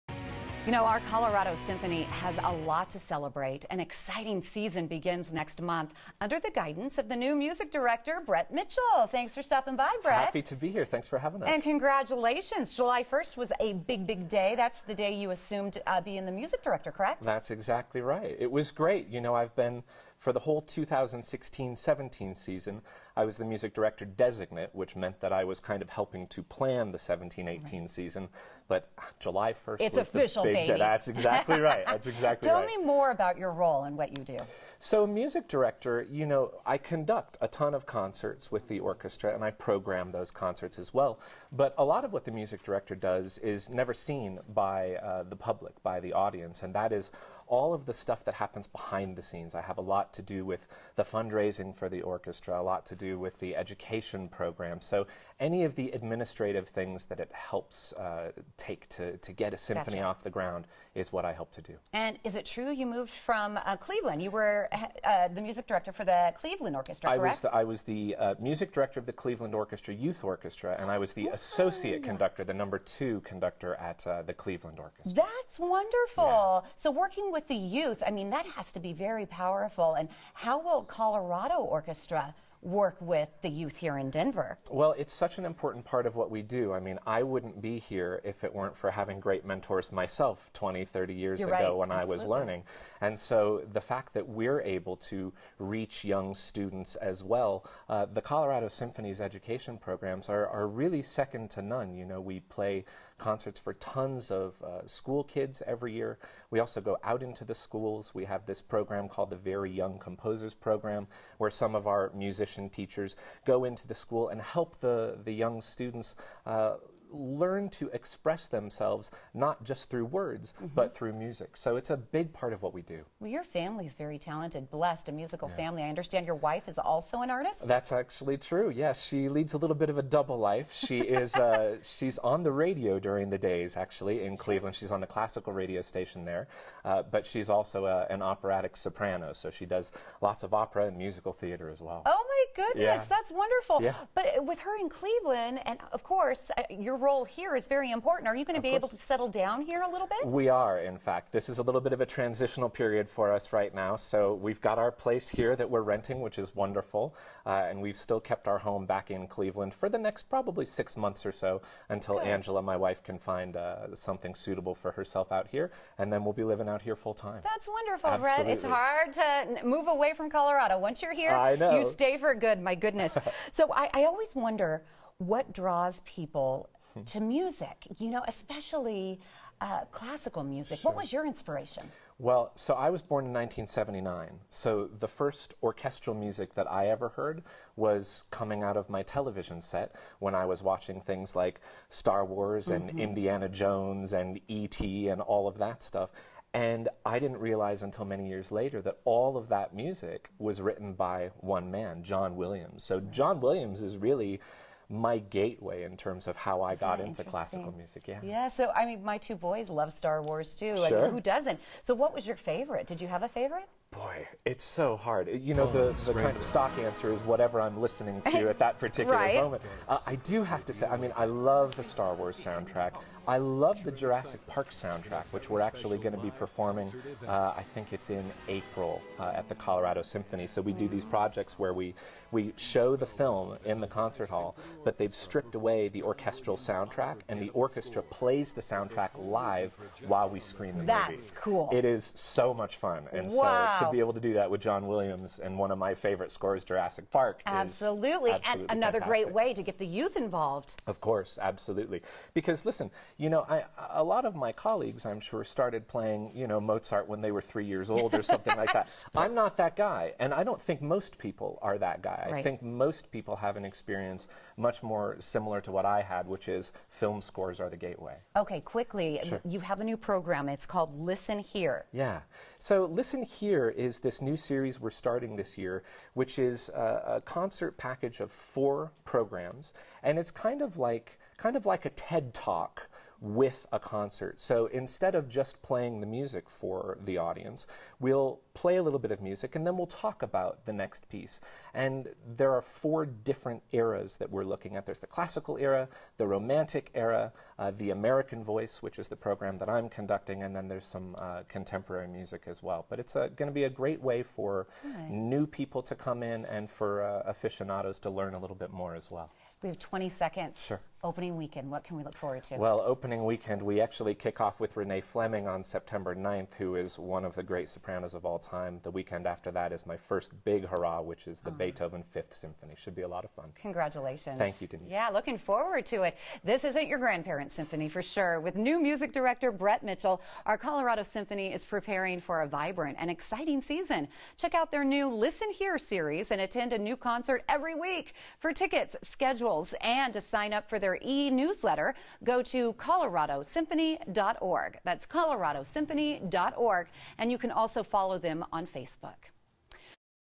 Colorado Symphony - August 24, 2017
This isn't your grandparents' symphony.  With new music director Brett Mitchell, our Colorado Symphony is preparing for a vibrant and exciting season.  Check out their new "Listen/Hear" series and attend a new concert every week. For tickets, schedules and to sign up for their e-newsletter, go You can also follow them on Facebook.